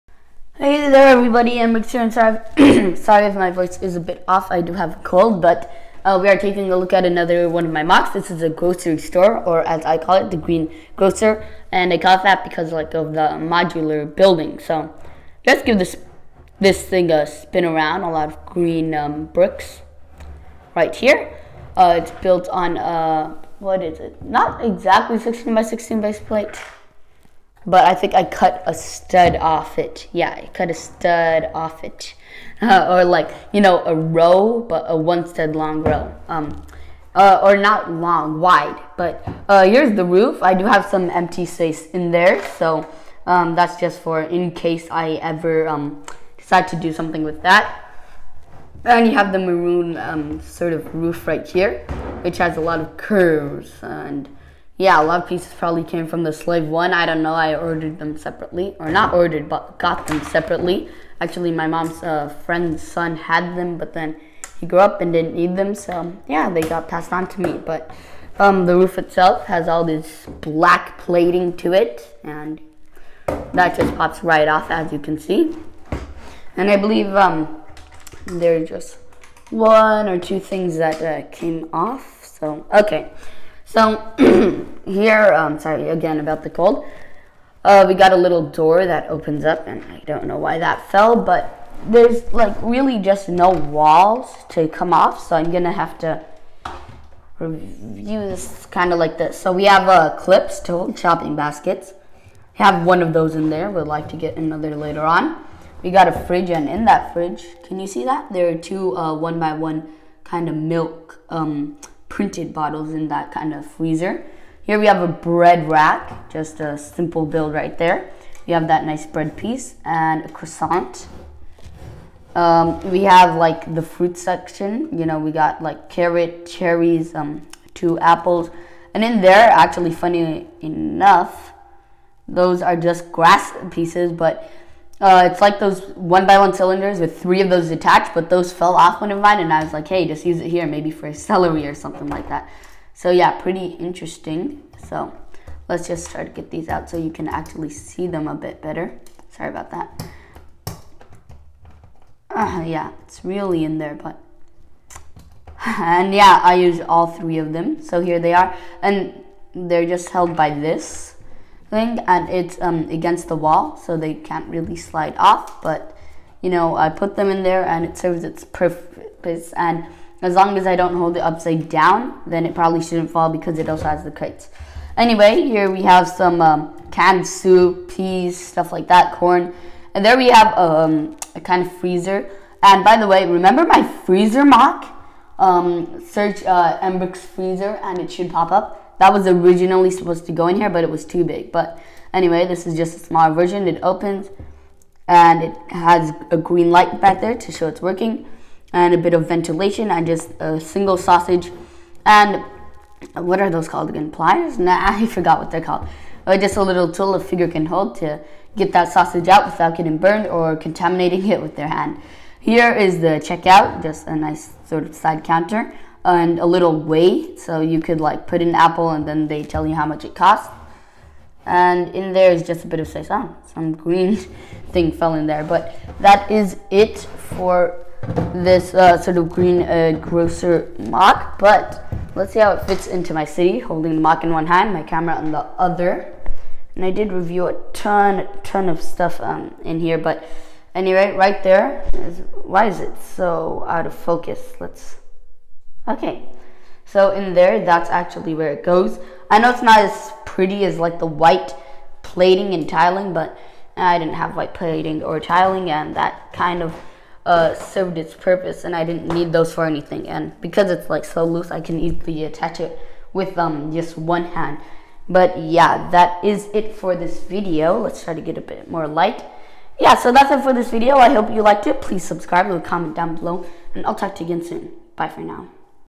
lego Grocery Store MOC! MOC City Update #10
Here's my moc grocery store, in titled The Green Grocer, get the reference?